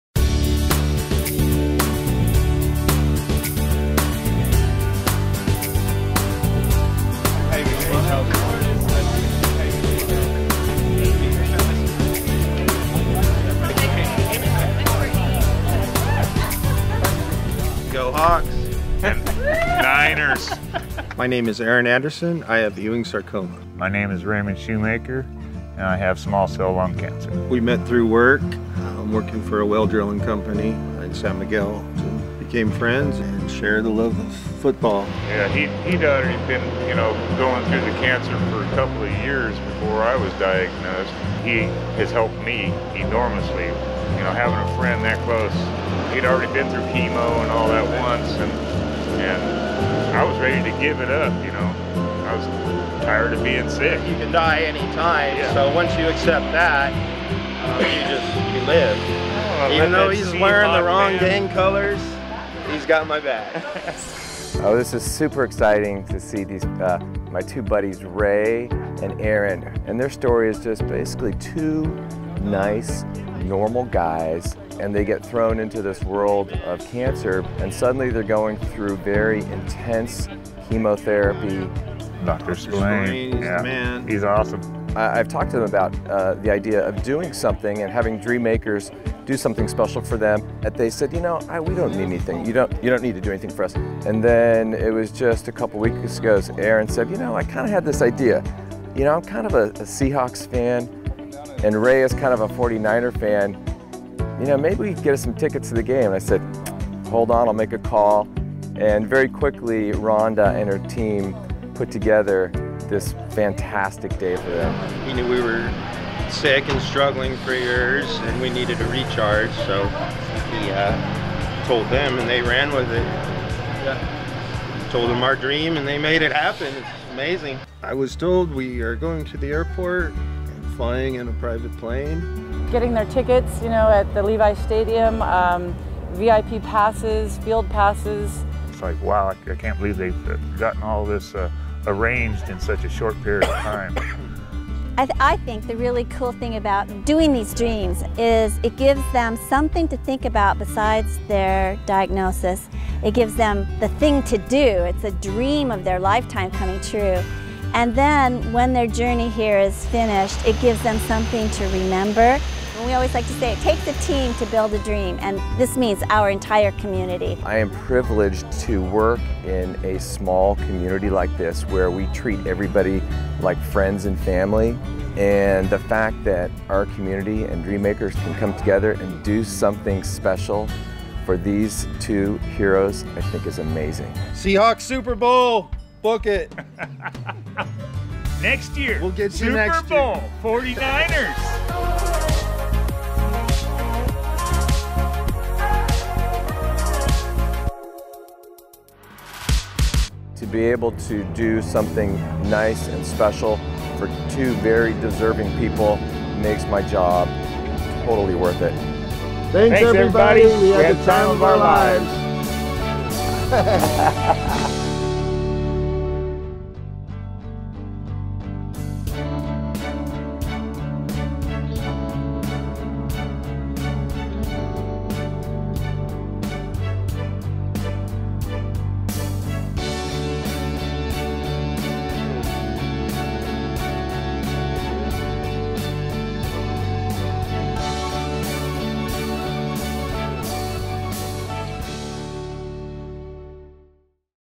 Dignity Health Central Coast - Football Dream
Two great friends, together on their cancer journey, enjoy a dream of a lifetime thanks to the generosity of Dignity Health, Pacific Central Coast Health Centers, Dr. Tom Spillane, SLO Oncology & Hematology, French Hospital Medical Center, ACI Jet and Dream Makers SLO.